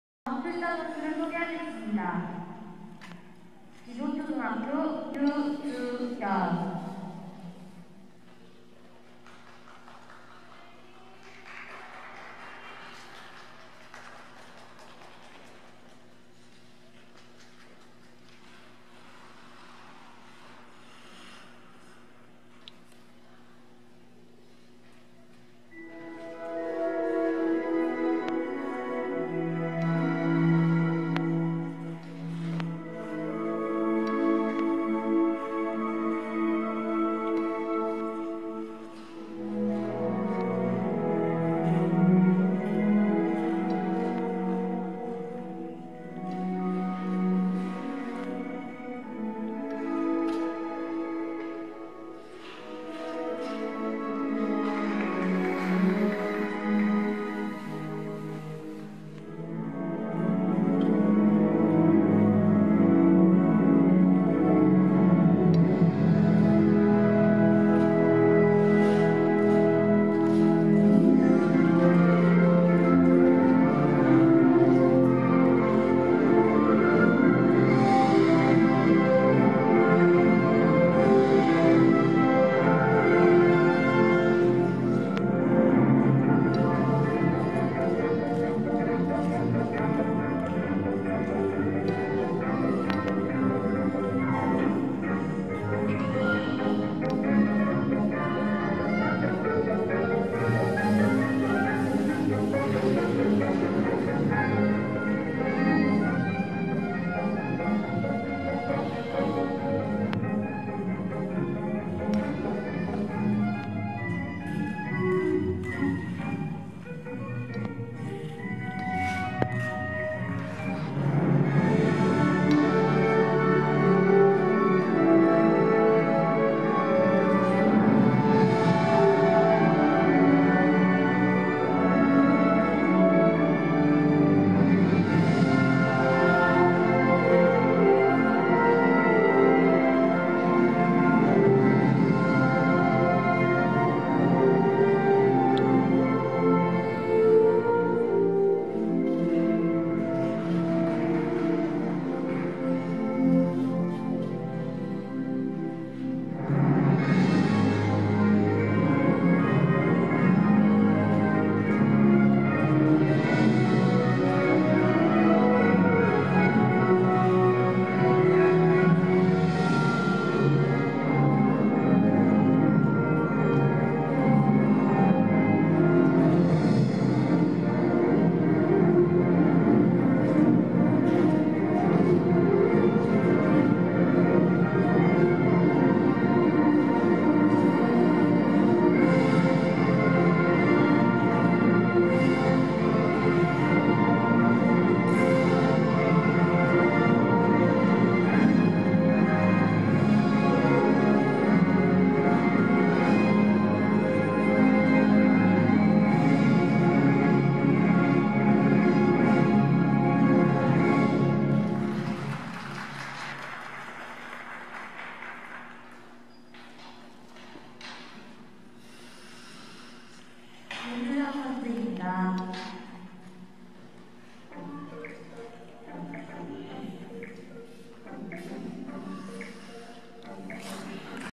유주연 | C조여초 FS 11 | 2018-12-09 동계체전 경기예선 | 4급 | 지도초
제5회 경기도회장배 피겨스케이팅대회 겸 제100회 전국동계체육대회 피겨스케이팅 경기도대표 선발대회 | 의정부 실내빙상장 |
Uijeongbu Indoor Ice Rink | Gyeonggi-do Figure Skating Union President Cup & Korea National Winter Sports Festival Gyeonggi-do Qualifire